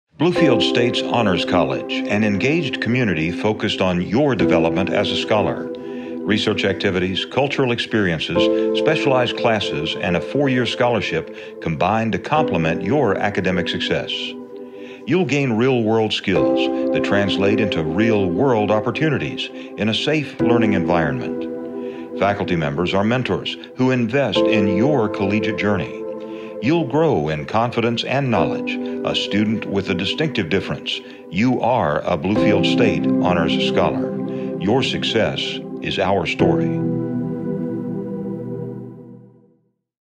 Bluefield State Honors College
The Bluefield State Honors College offers an exciting opportunity for high achieving students interested in an advanced level of academic engagement both inside and outside the classroom, making them more prepared and competitive in their future graduate studies and careers.